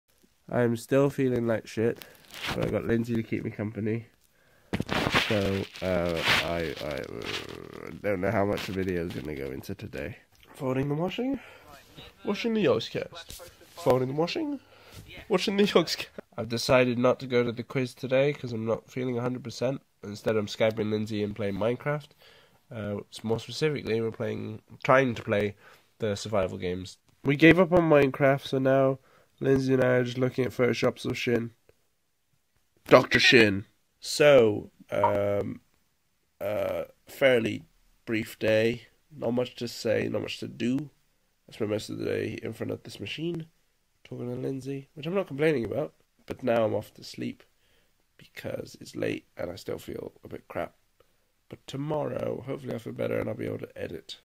Still Ill - 26/11/2013
So after a sleep and a day and work. I'm still ill, but luckily I had Lynsey, Photoshops of Sjin and Minecraft to keep me company